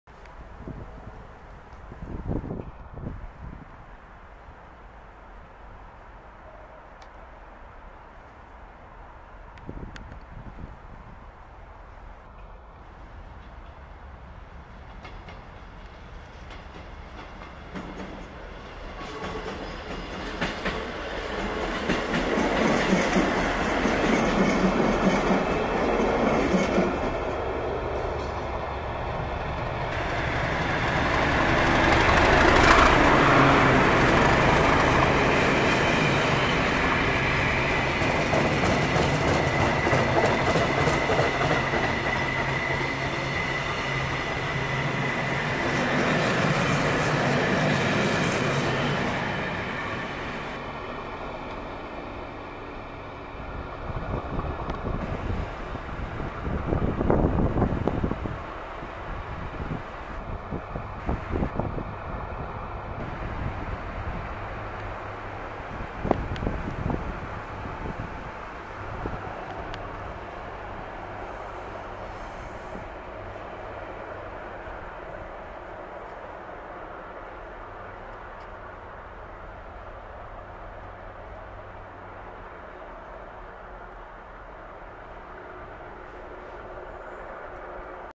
37800 passes Darlington - 5L46 Haymarket Depot to Ely Mlf Papworth Sidings
37800 - ‘Cassiopeia’

TF: 41136
TS: 42207
TS: 42047
TGS: 44015